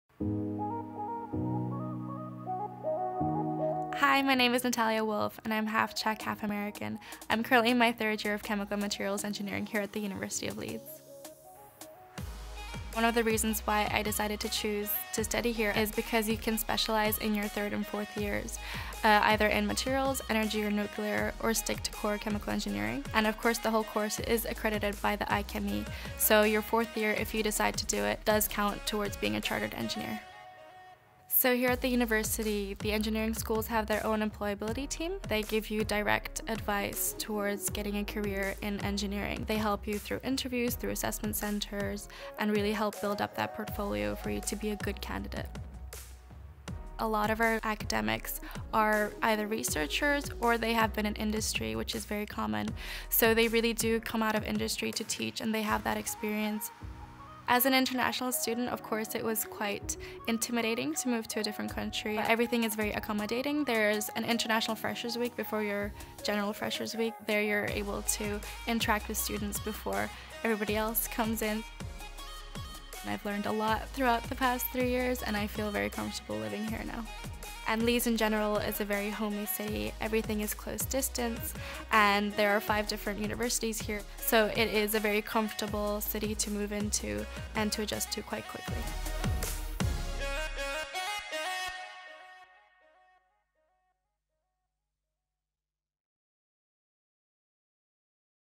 Student experience - Natalia Wolfe - Chemical and Materials Engineering MEng, BEng
Natalia Wolfe discusses her experience of studying Chemical and Materials Engineering MEng, BEng at Leeds.

Find out more: eps.leeds.ac.uk/chemical-engineering-undergraduate